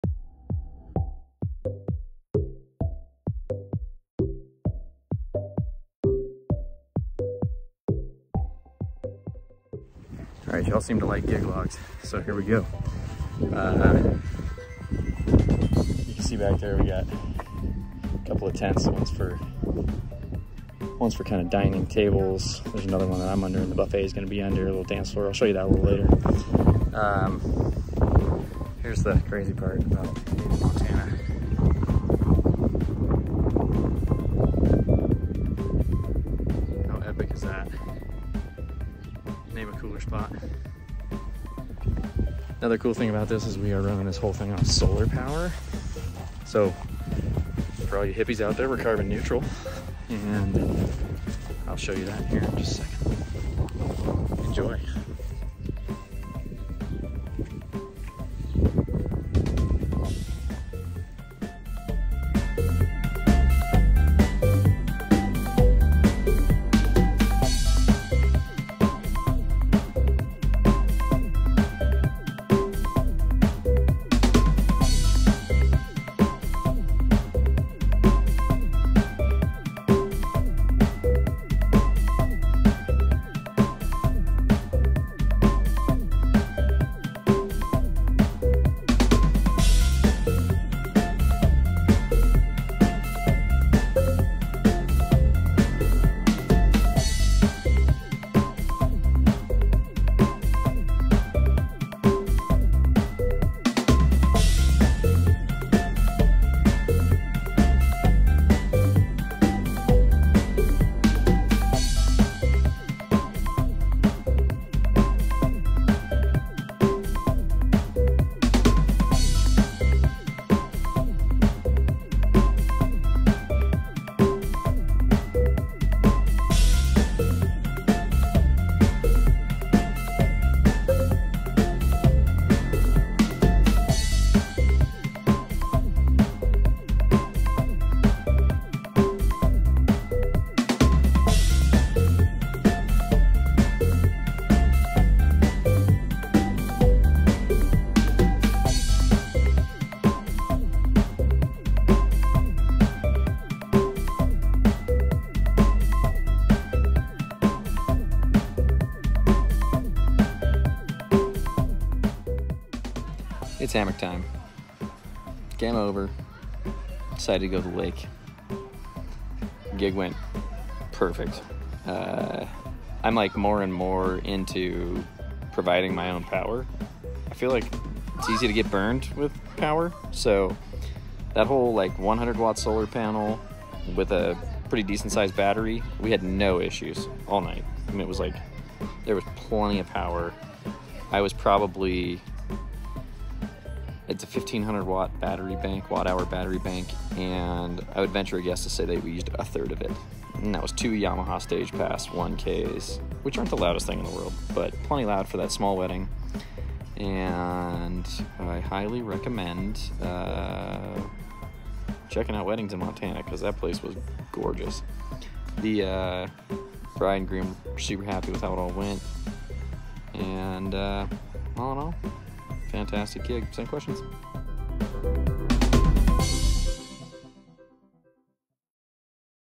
Gig Log 1 - Solar power only and some epic mountains.
Mission Mountain wedding in Montana with power provided by a 100w solar panel and a Bluetti EB150 battery generator. Ceremony sound powered by an Ecoflow River Pro battery generator. Denon DJ Prime 4, and Prime Go. QSC CP8 for ceremony, 2x Yamaha STAGEPAS 1k speakers for reception. Epic night, great wedding to DJ. Just please God, no more Abba requests!